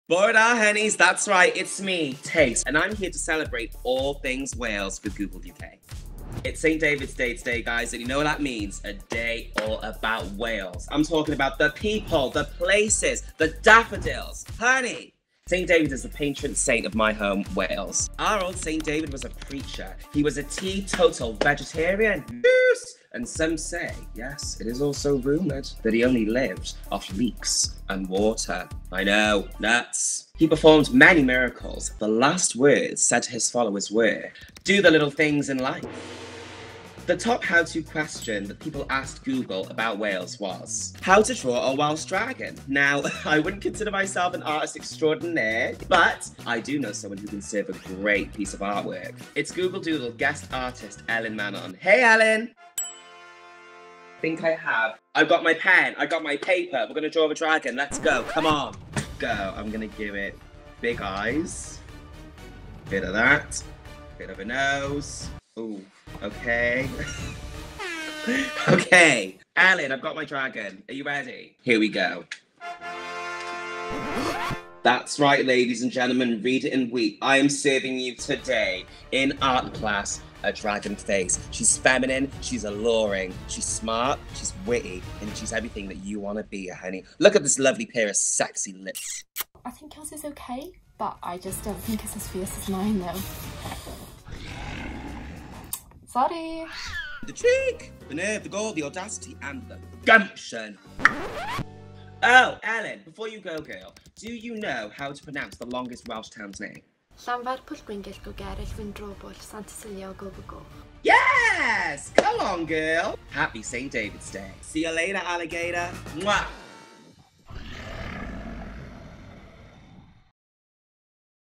St David's Day with Tayce | Patron Drag Queens of the UK | Google UK
RuPaul's Drag Race UK's Tayce takes a little look at some of the top Searches about Wales over the last year for St David's Day 2021.

Featuring Elin Manon, Illustrator and Google Doodle guest artist.

goo.gle/st-davids-day-2021